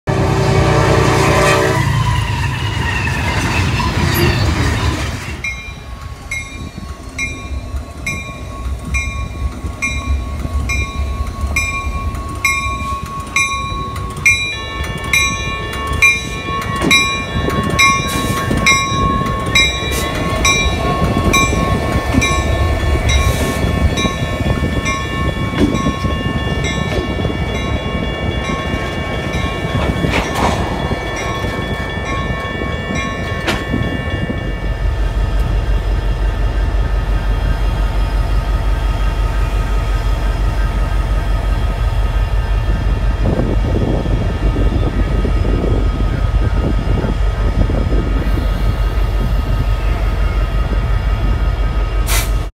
Safetran Type 1 Bell in Action
This Safetran type 1 in action at the Cemex plant while it gets switched by the LSF51